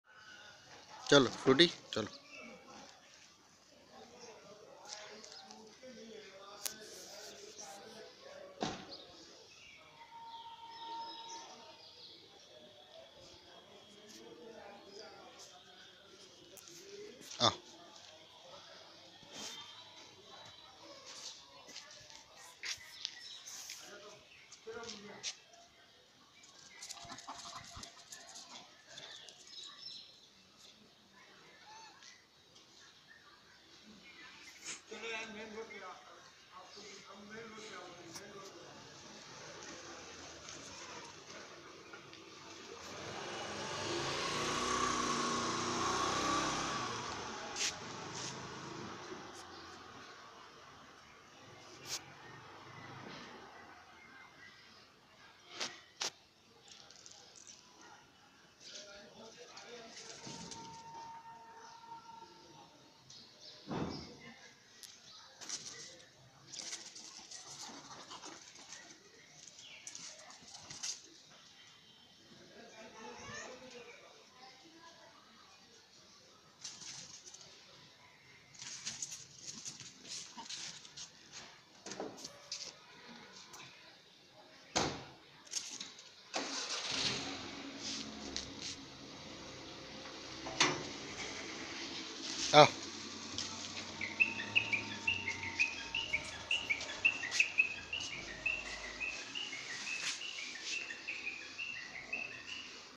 Pug vs Monkey | Monkey climbed on her back 😂😂 | My first pug video (Frooty) ....
please support 😊